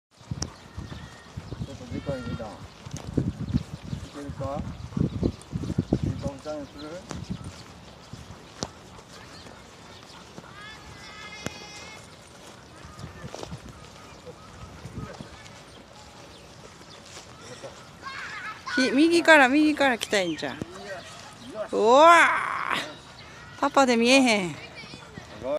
Danke☆ 切り株チャレンジ♪成功♪♪ in 浜寺公園
ワイマラナー＆E・コッカー・スパニエル Mix犬

【アルバム】浜寺公園は切り株がいっぱい♪